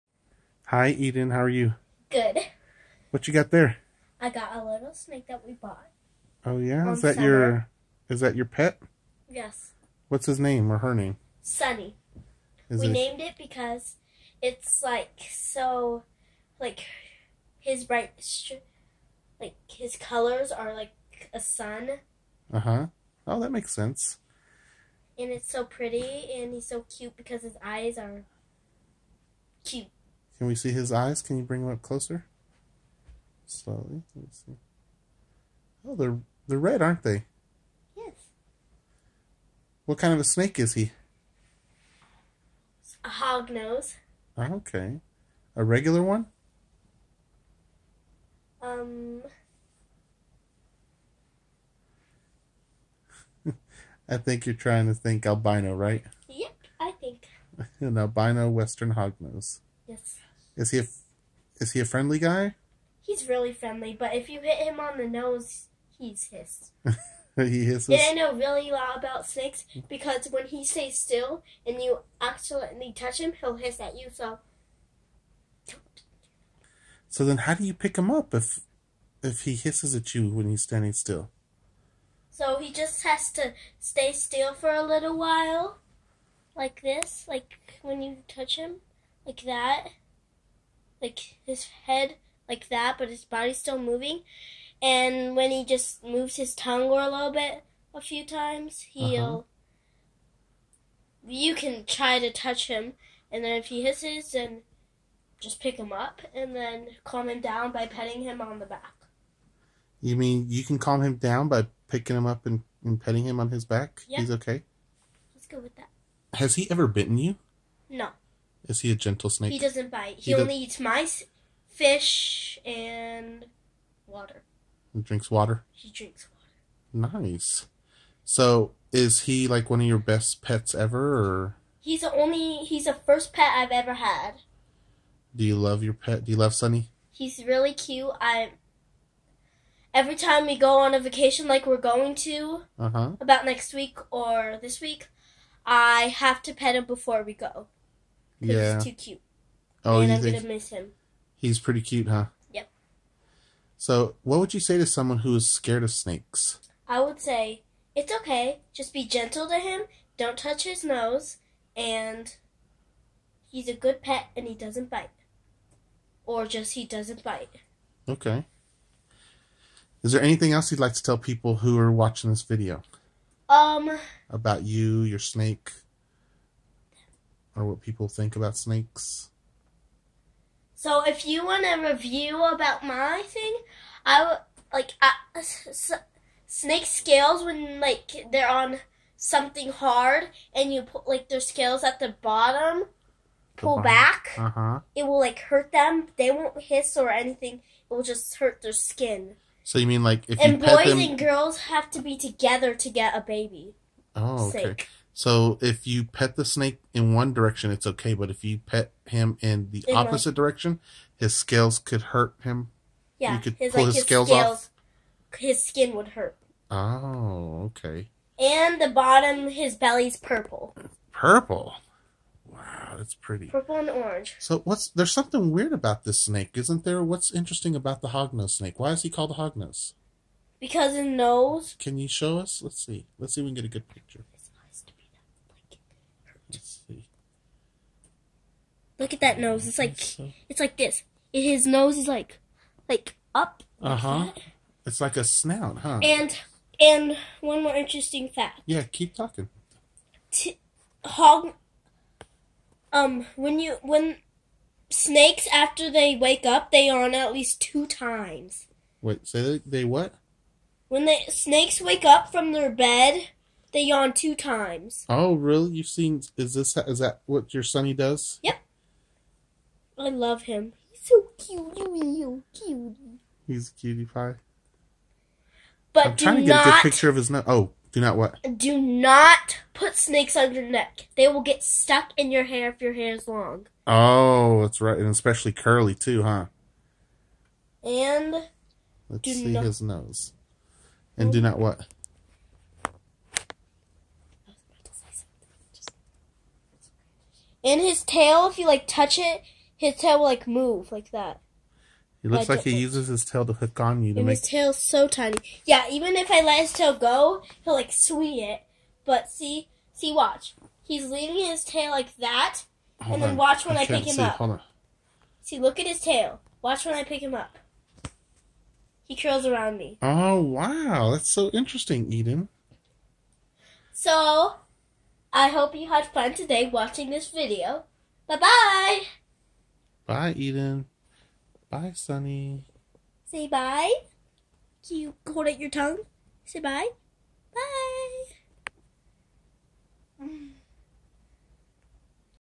Young child being interviewed about her pet albino western hognose
Asking my daughter about her pet hognose, Sunny. Fun video interview.